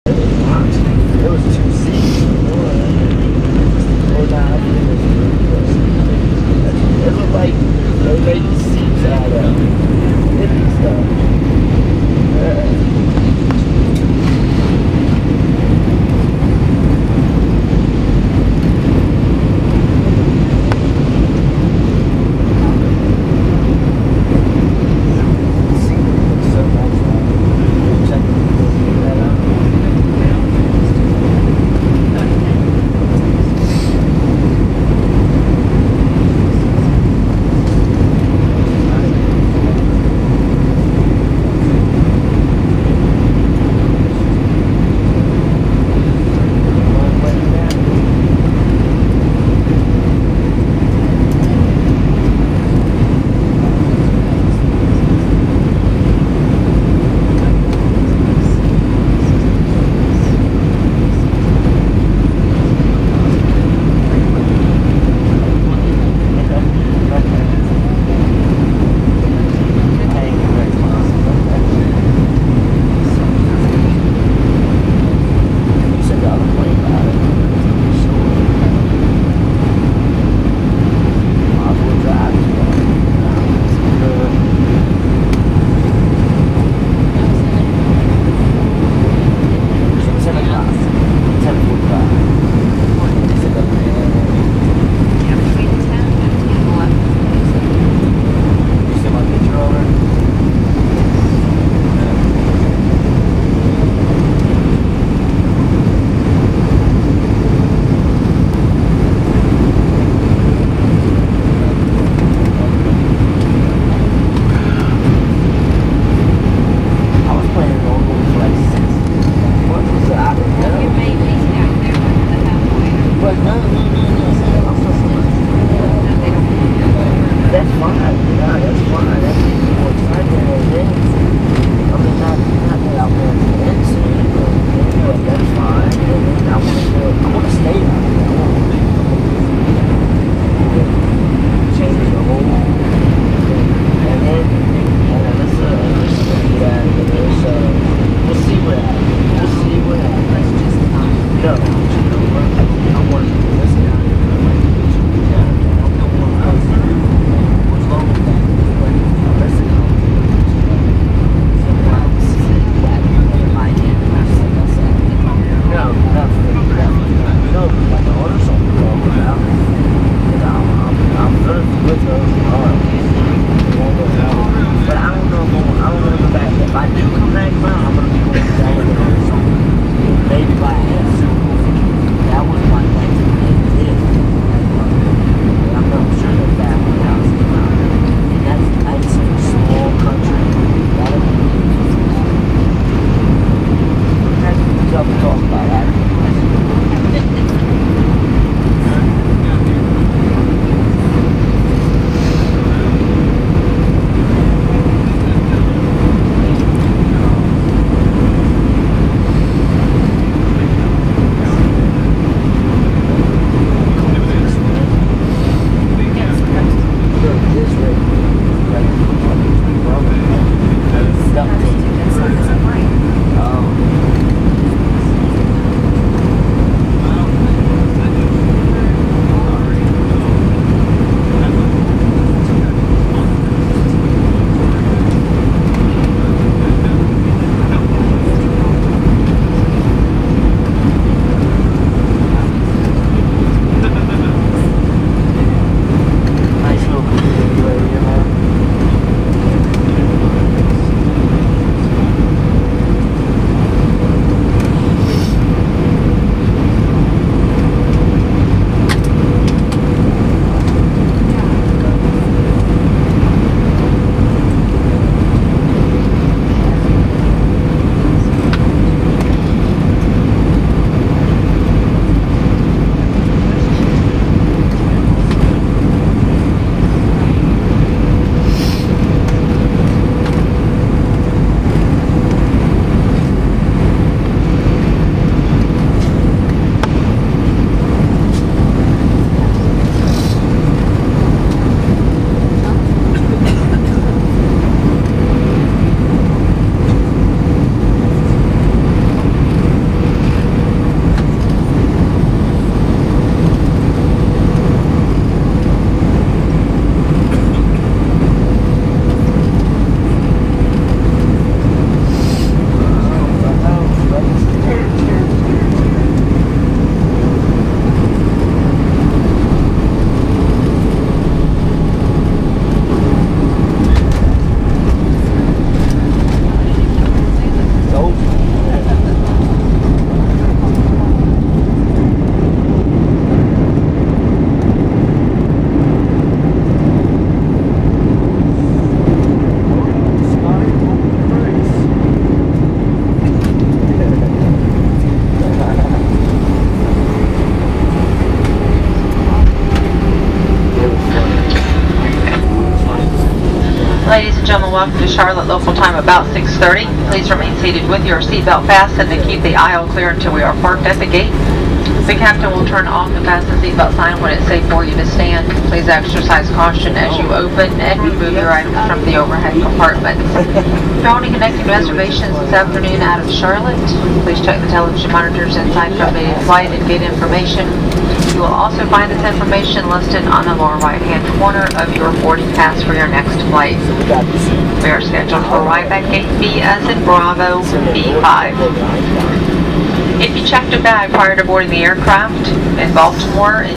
Beautiful US Airways A320 Landing in Charlotte International Airport (CLT)
US Airways A320 landing in Charlotte. The flight originated in Baltimore-Washington International Airport (BWI)